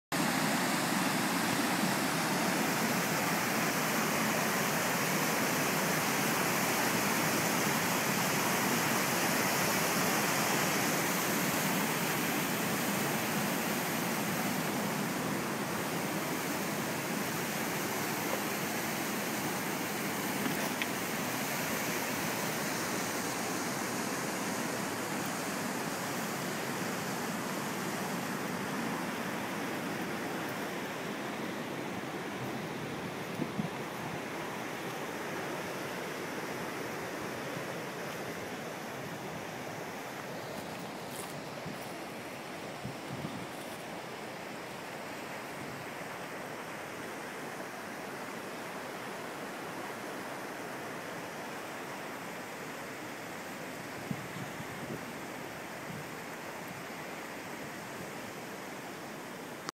Rural Iceland (part 1)
a short video filmed in the valley near Dalvik in northern Iceland